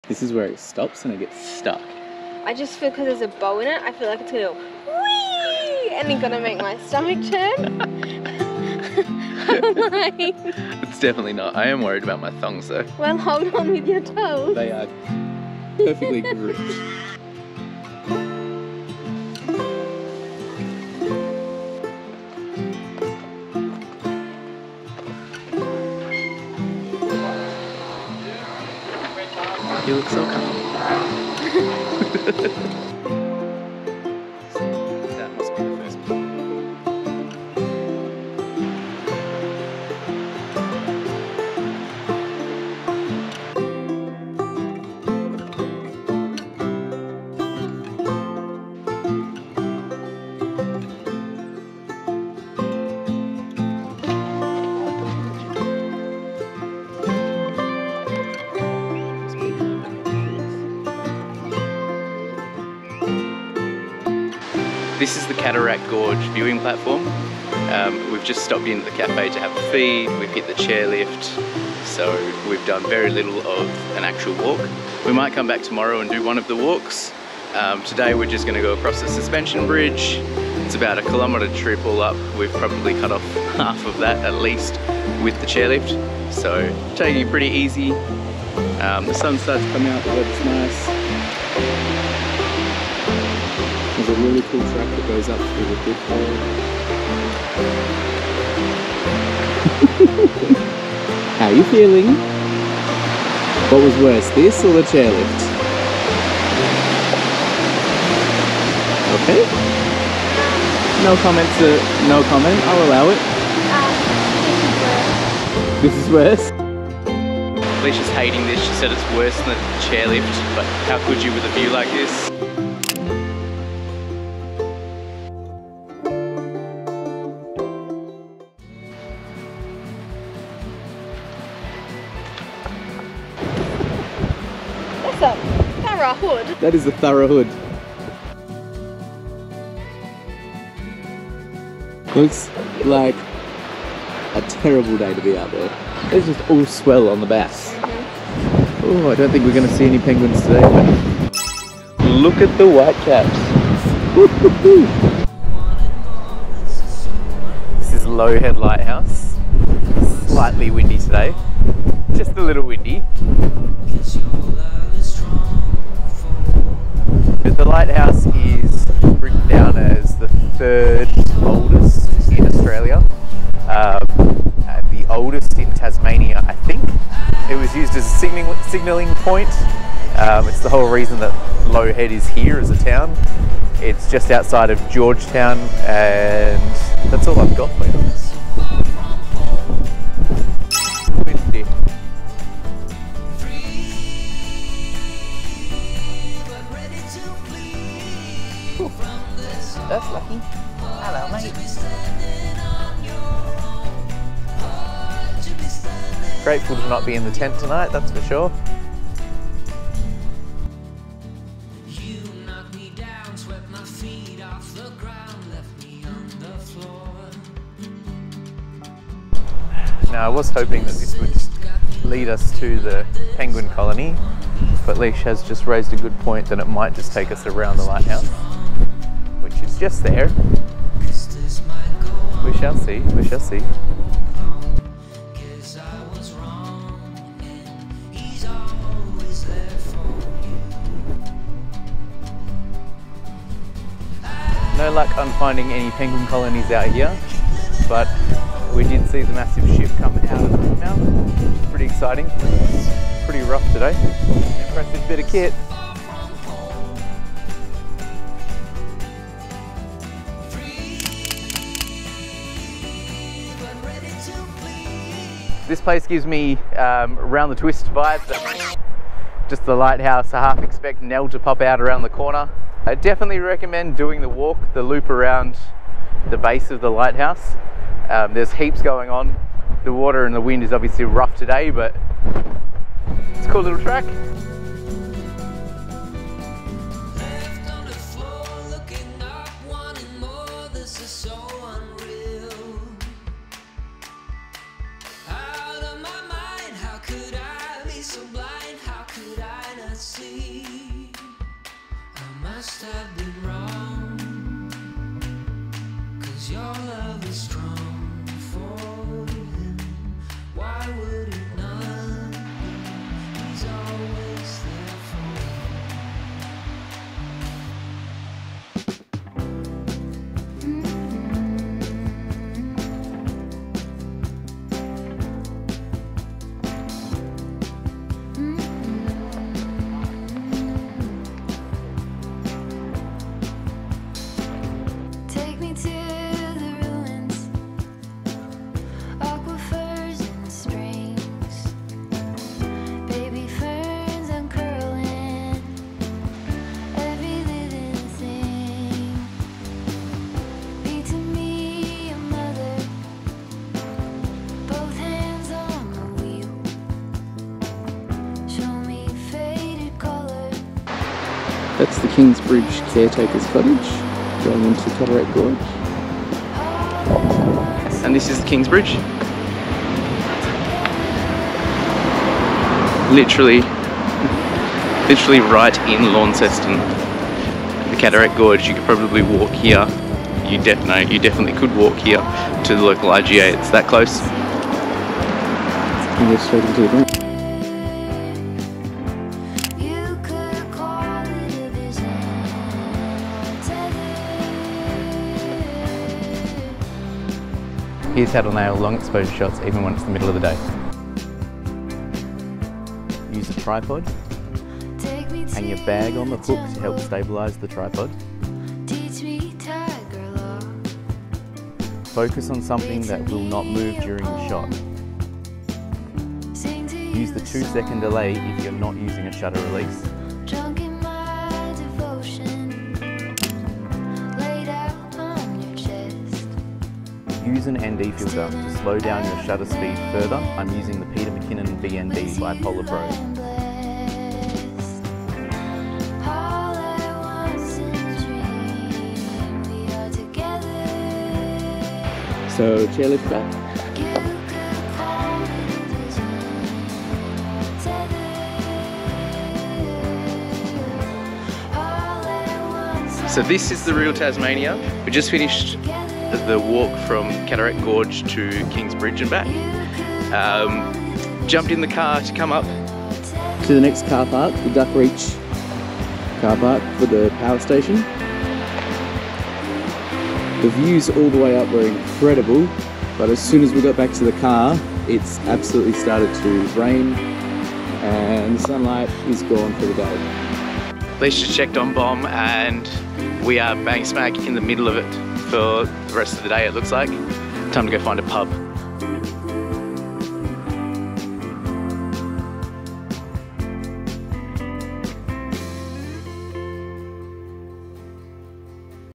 10. Adventures at Cataract Gorge & the windswept Low Head. (Our Lap of Tasmania)
Our lap of Tasmania continues; time in Launceston leads us to making the most of good weather to explore Cataract Gorge. We completed two of the walking tracks seeing both the suspension bridge and Kings bridge. There was also some cheating by taking the chairlift to the cafe.

A morning drive took us out to a very windy Low Head where we unsuccessfully searched for penguins and learnt about the second oldest lighthouse in Tasmania and Australia's oldest continuously used pilot station.

Learn tips to nail your long exposure shots especially in daylight.
Camera Gear Used:
Canon 90D (@canonaustralia)
Polar Pro x Peter McKinnon VND 2-5 (@PolarProFilters @PeterMcKinnon )